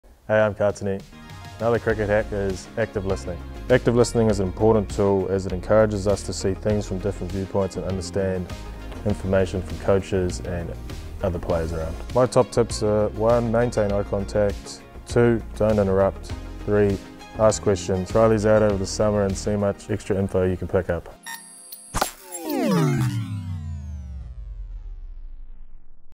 CricketHacks | Active Listening
Part of Smash Play youth-rangatahi from NZC. Smash Play Ambassador and Northern Brave batter Katene Clarke talks through the importance of active listening.

Find more engaging content from Callum, Prue, Katene and Ocean at the following